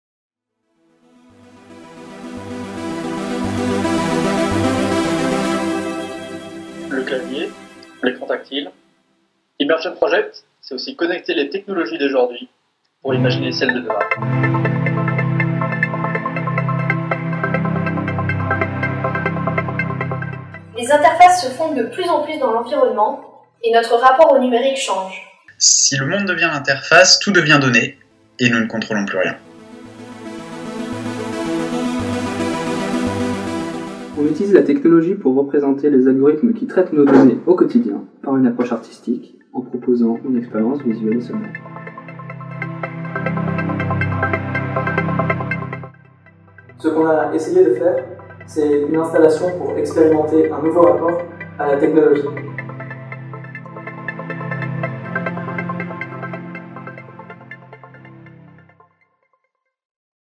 Pré-Pitch IMMERSION Project
Droits d'auteur :
 - à l'exception de la 1ère vidéo (0-3s, mère & enfant), libre de droits ;
 - les musiques, les vidéos, sont la création de notre équipe.

Présentation de IMMERSION Project - en 1minute - pour la finale Coup2Boost.